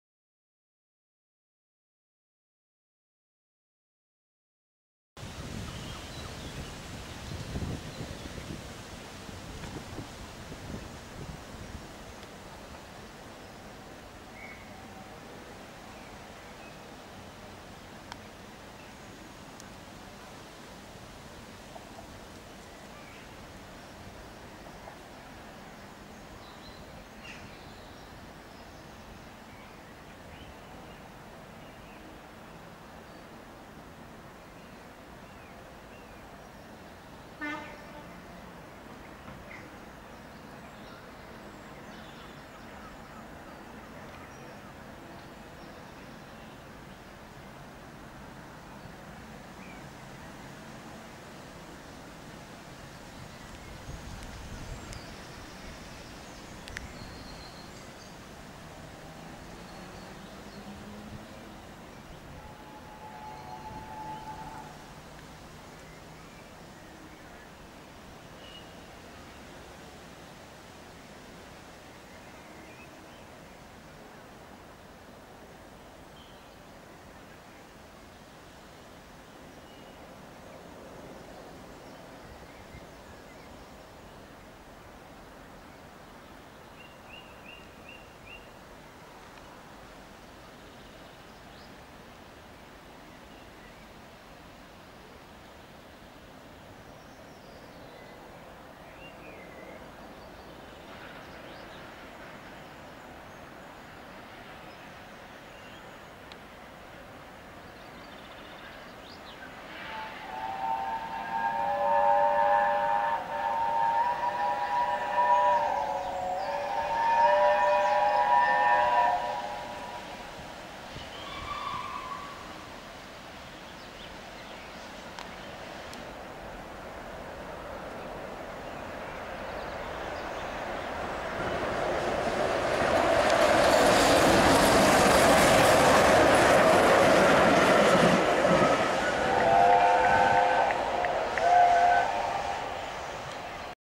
60163 Tornado Totnes 10 June 2017
A wet mist was blowing up the Dart but I managed to find a fairly sheltered spot on the path to the South Devon Railway's Totnes Riverside station to film from.

Lovely use of the chime whistle through Totnes station and appreciative applause from Riverside at the Tornado fly past.